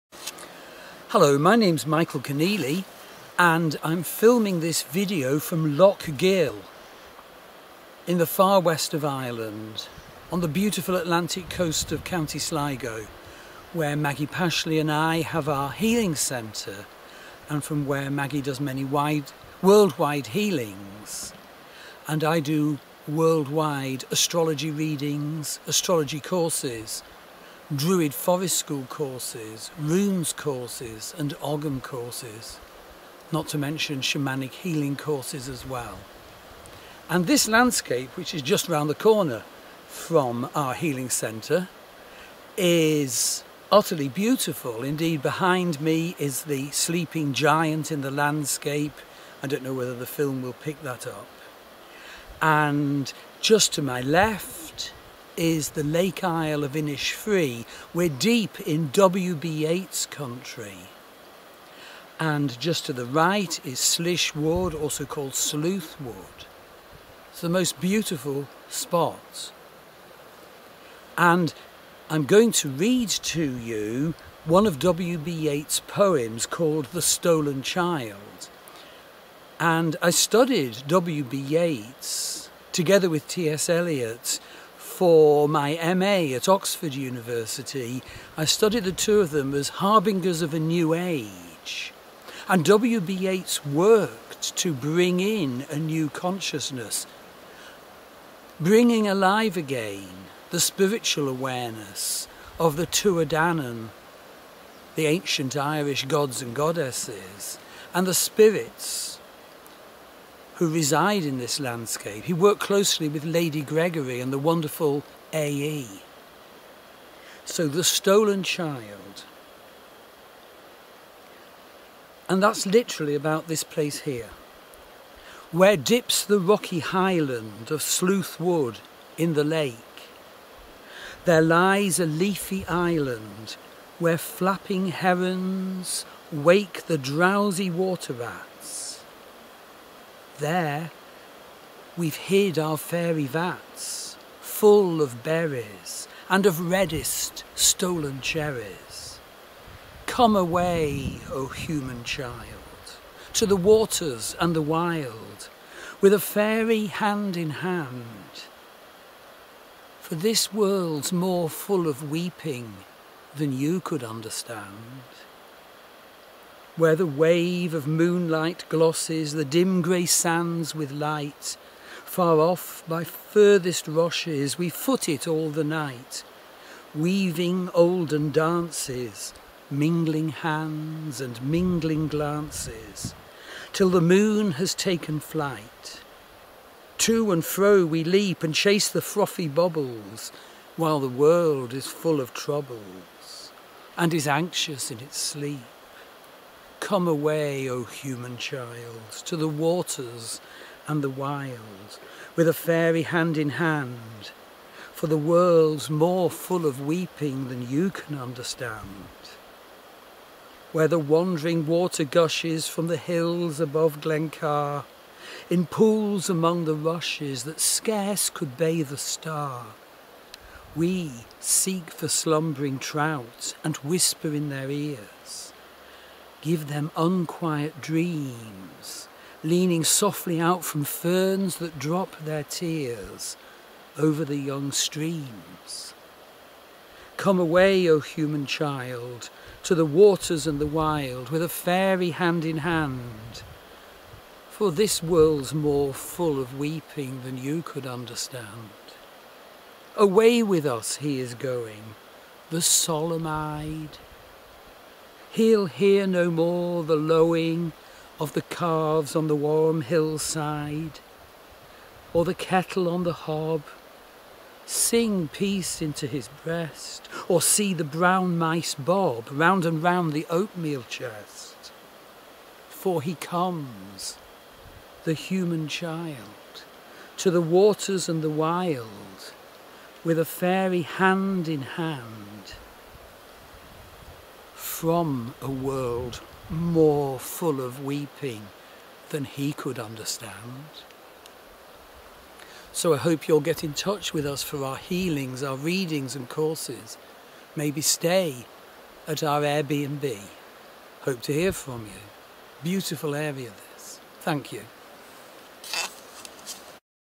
W B Yeats The Stolen Child and Sacred West of Ireland Tours
This reading of W B Yeats The Stolen Child is filmed at Loch Gill, County Sligo in the far West of Ireland. To join this very special magical, poetic and visionary tour of beautiful Loch Goll in Sligo in the West of Ireland: